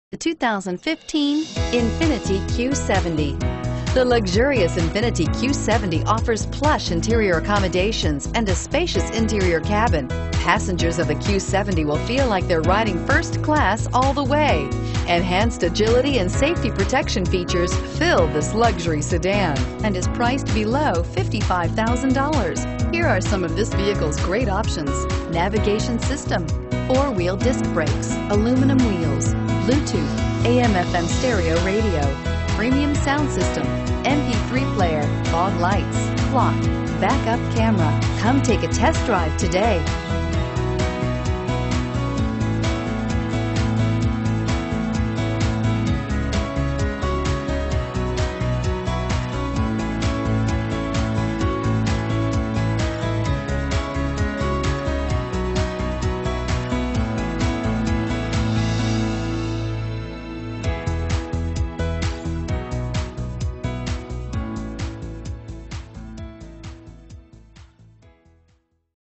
2015 Infiniti Q70 Houston TX Q7B069
2015 Infiniti Q70

We are pleased to present this excellent 2015 Infiniti Q70 with 0 miles. Its beautiful Black Obsidian exterior is nicely complimented by a clean Graphite interior.  This Q70 comes equipped with the following options,  , 5006# Gvwr, Chrome Door Handles, Bluetooth Wireless Phone Connectivity, Perimeter/Approach Lights, Front Windshield -inc: Sun Visor Strip, Rain Detecting Variable Intermittent Wipers, Double Wishbone Front Suspension w/Coil Springs, Chrome Grille, Fixed Rear Window w/Defroster,  and much more. For a complete list, select the Options button below.